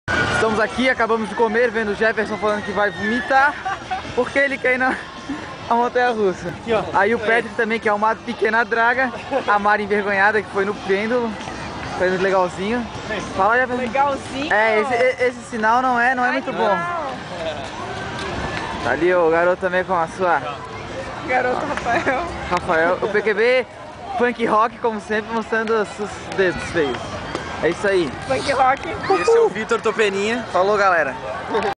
Hopi Hari
Depois do Hadikali e de bater um ranguinho no Hopi Hari